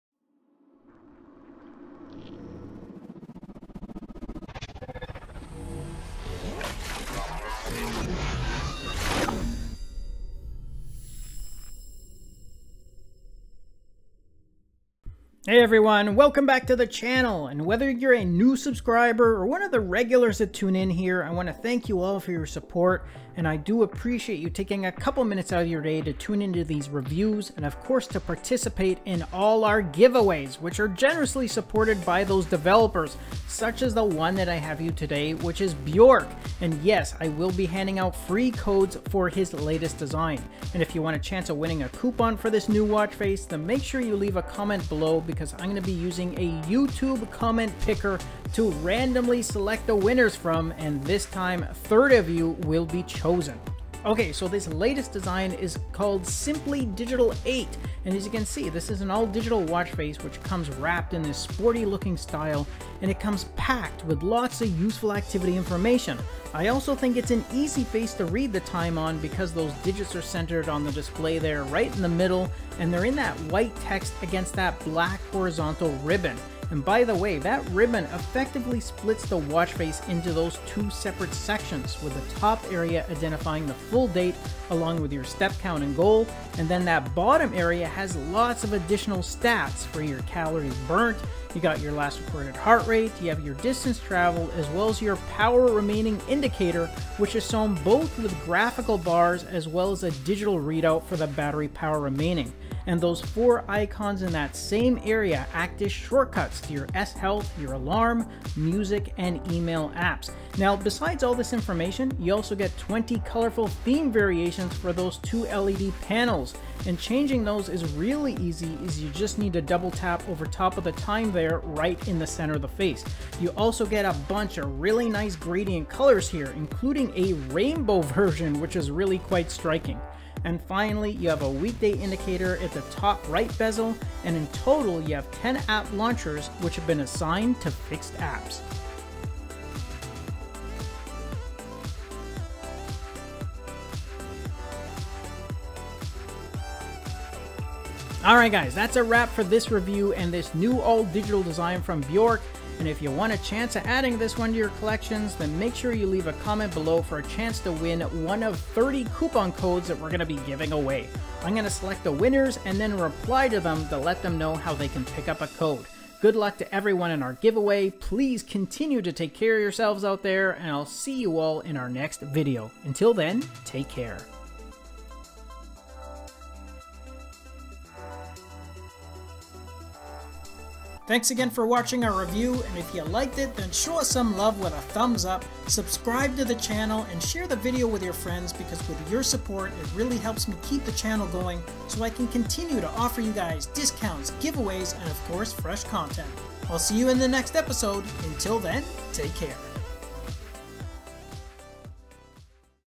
Limited Codes to Giveaway! Samsung Galaxy Watch Active 2/Galaxy Watch Faces - Jibber Jab Reviews!
30 coupons will be given away to random and lucky viewers of the channel!  Leave a comment below in order to participate and if you are selected as a winner I will leave a reply to let you know how you can get the code!

Samsung Watchfaces
Smartwatch